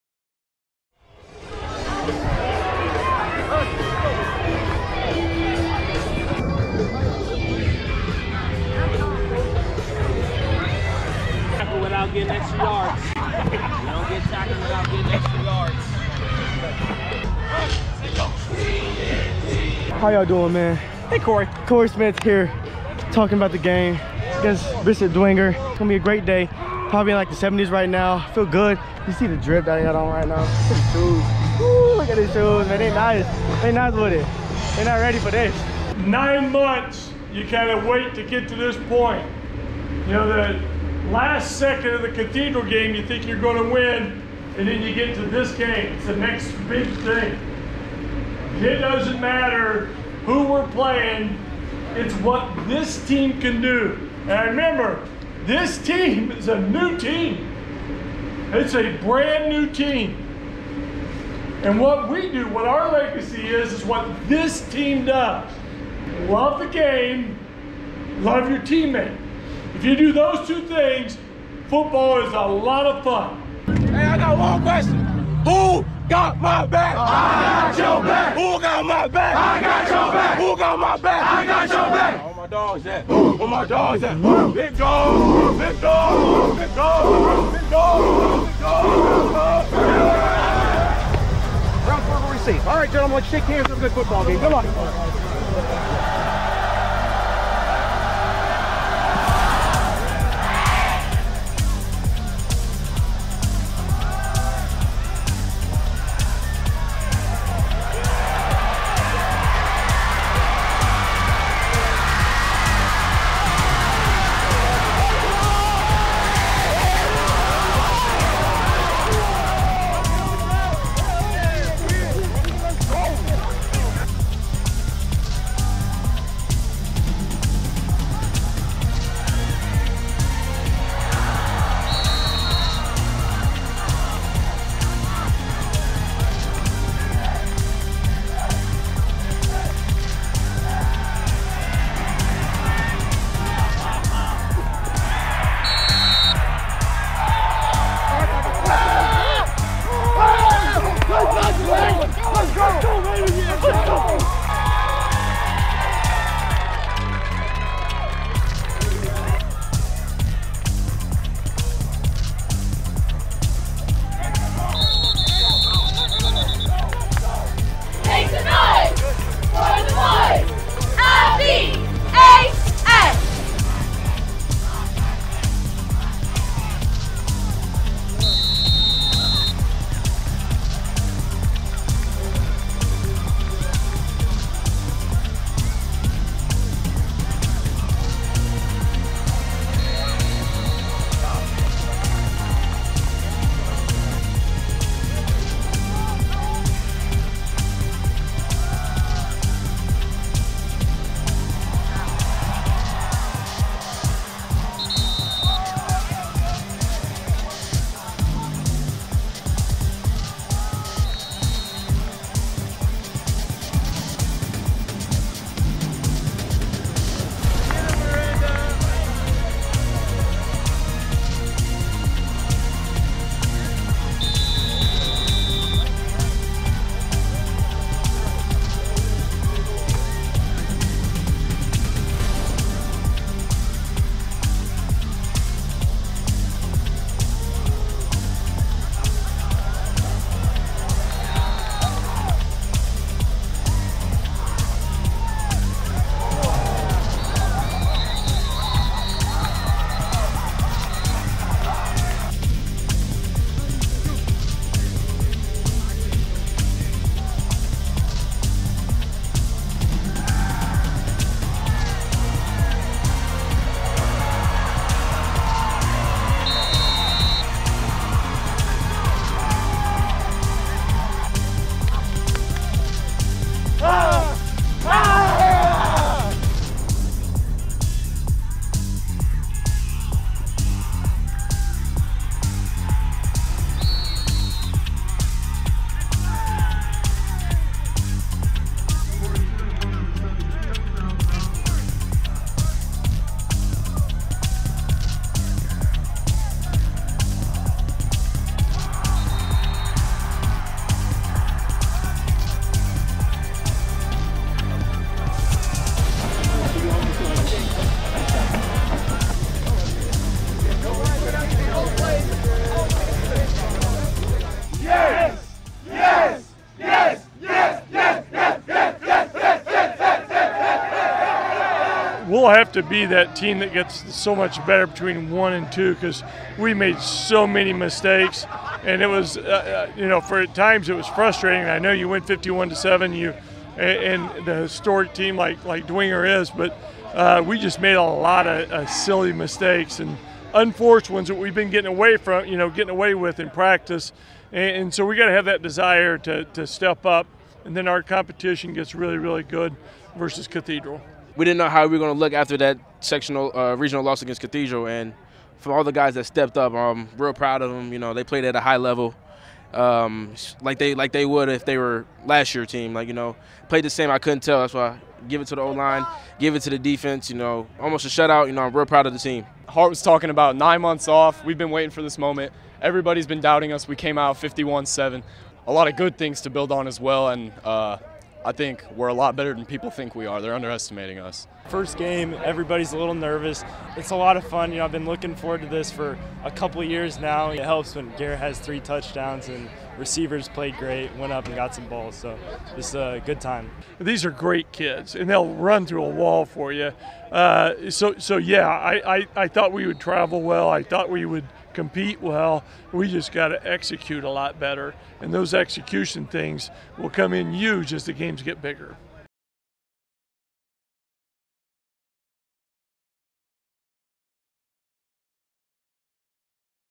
2023 Week 1 SIGHTS & SOUNDS: Brownsburg at FW Bishop Dwenger
Video courtesy: Brady Morse, BHS-TV
Relive Brownsburg's 2023 season opening victory at Fort Wayne Bishop Dwenger with this week's Sights & Sounds!
The Dogs return to action on Friday, August 25th when they host Cathedral in their home opener of the 2023 Indiana high school football season.